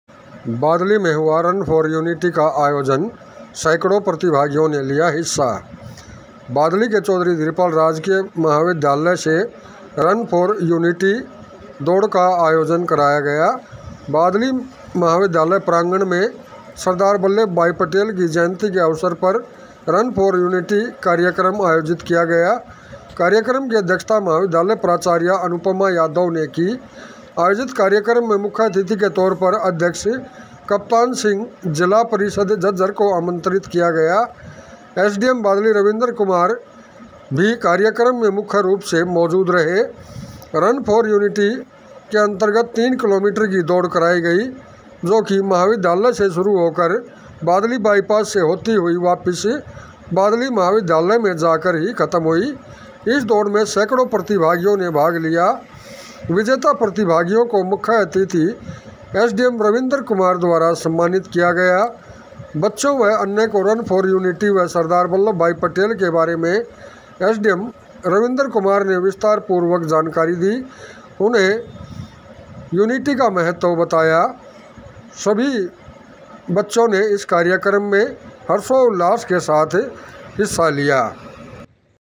बादली में हुआ रन फॉर यूनिटी का आयोजन
बादली के चौधरी धीरपाल राजकीय महाविद्यालय में रन फॉर यूनिटी का आयोजन किया गया। महाविद्यालय बादली प्रांगण में सरदार वल्लभभाई पटेल की जयंती के अवसर पर रन फॉर यूनिटी का आयोजन किया गया। कार्यक्रम की अध्यक्षता महाविद्यालय प्राचार्या अनुपमा यादव ने की। आयोजित कार्यक्रम में मुख्य अतिथि के तौर पर अध्यक्ष कप्तान सिंह जिला परिषद झज्जर को आमंत्रित किया गया। एसडीएम बादली रविंद्र कुमार ने भी कार्यक्रम में मुख्य रूप से शिरकत की।
Recent videos
Khap Panchayat
Wrestlers Protest